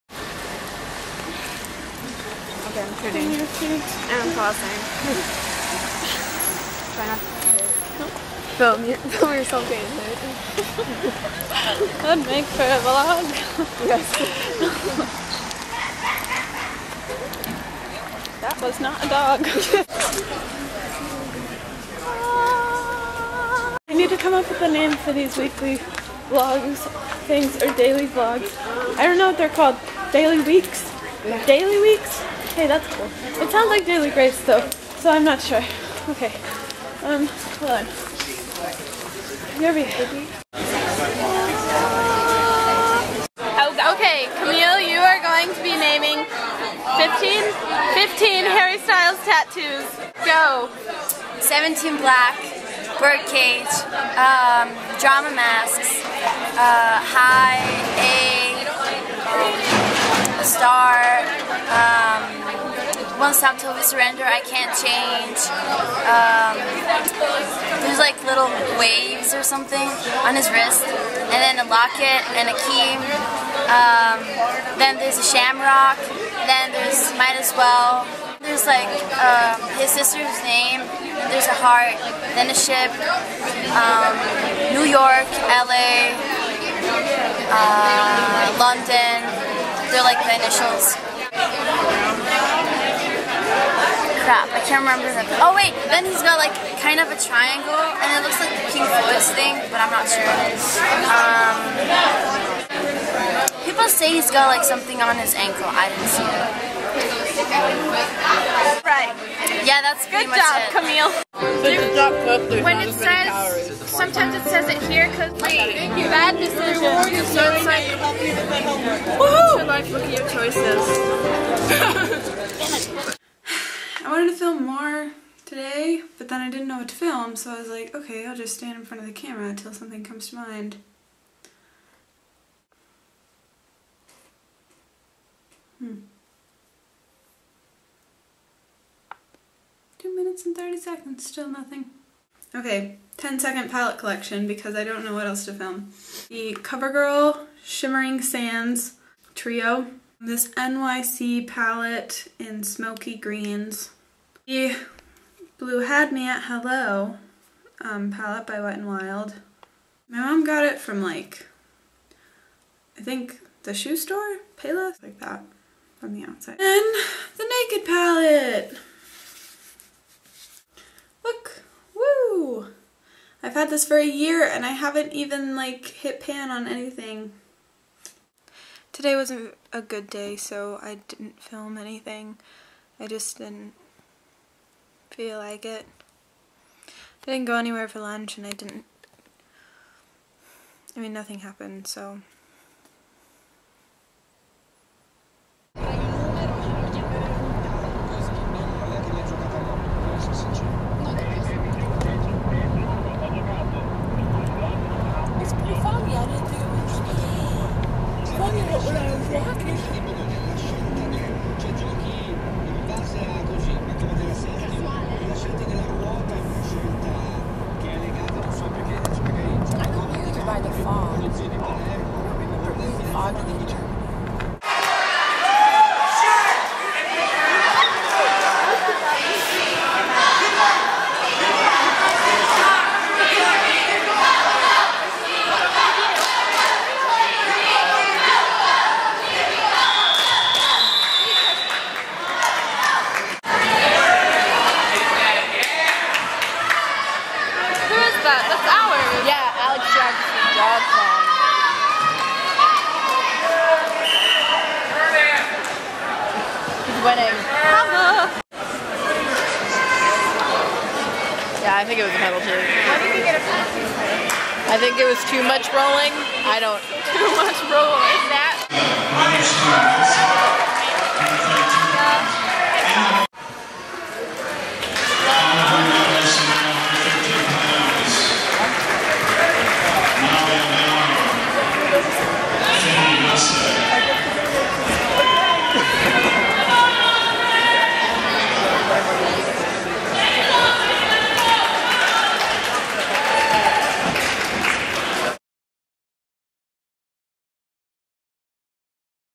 Riki's Weeklies vol. 2
This week I did a lot of thinking and I've decided that the new name is going to be Riki's Weeklies because it makes sense. This episode involves friends, sadness, and wrestlers so yay! Enjoy!